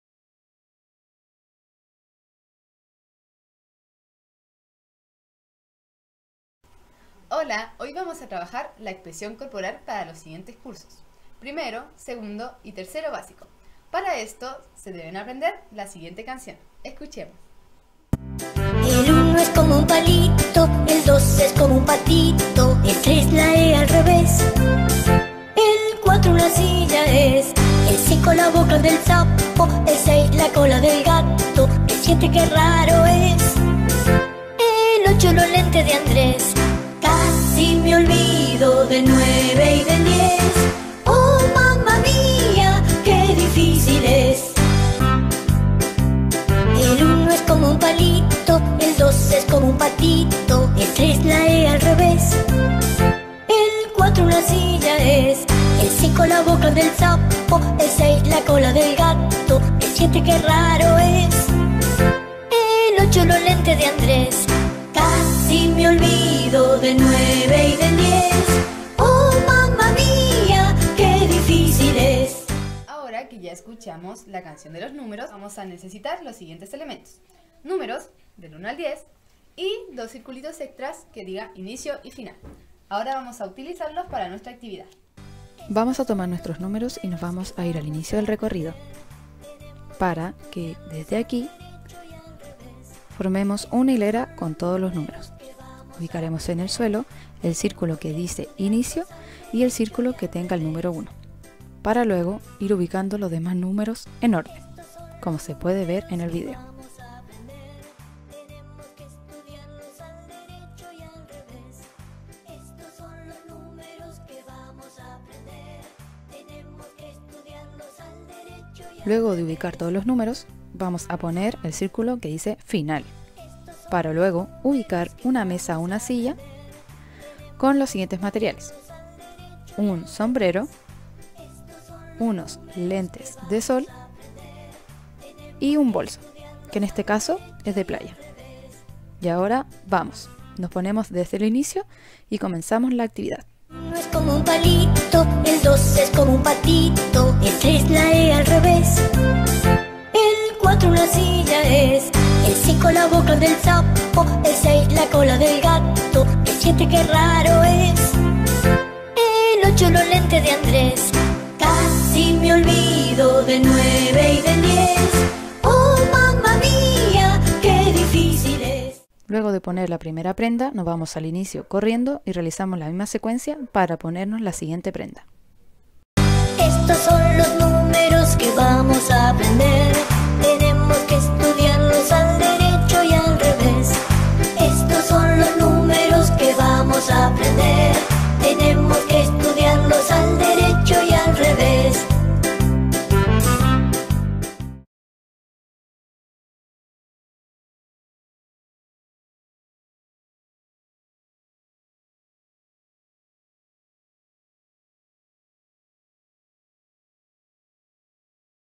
Expresión Corporal II | 1° a 3° básico
Juegos para el desarrollo de habilidades motrices de expresión corporal en niños de 1º a 3º básico (6-8 años), para realizar de manera entretenida en familia. creado por las profesoras de educación física en formación, Fernanda Pávez y Estephany Pávez, de pedagogía en educación física de la Universidad San Sebastián.
Vídeos realizados por estudiantes dela carrera de Pedagogía en Educación Física de la Universidad San Sebastián, en el marco del trabajo colaborativo que se realiza en el programa